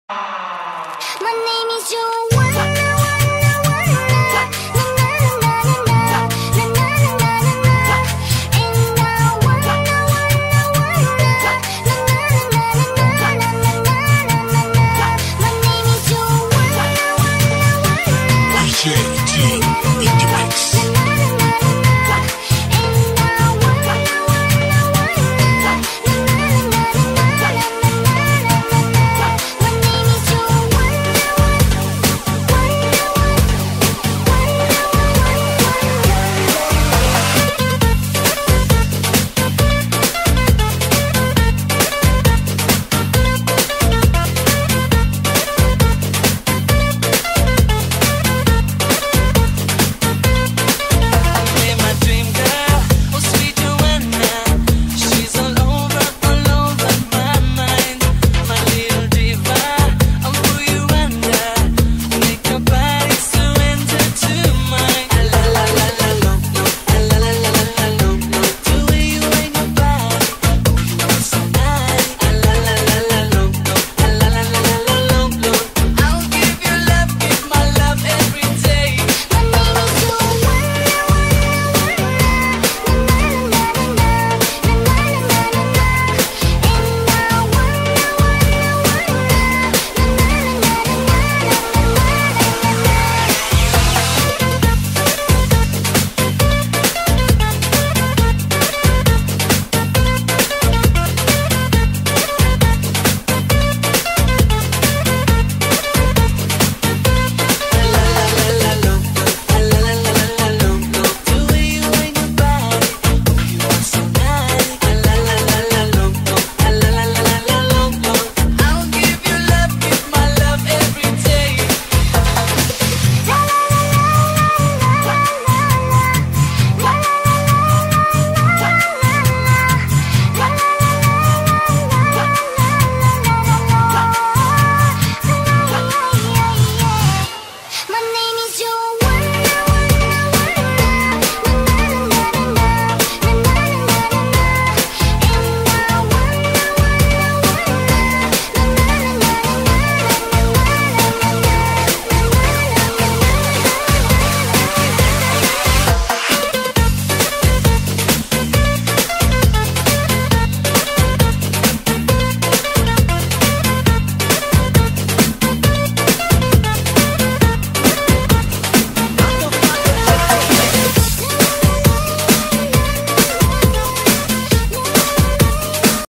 Laem Chabang Car Audio Show with Coyote Dancers 2012 File 03
Car Audio Show with coyote dancers held at the Harbor Mall Laem Chabang Thailand - music remix by DJ Teen Pattaya - video taken on 8 August 2012 with my Sony HDR Digital Camera.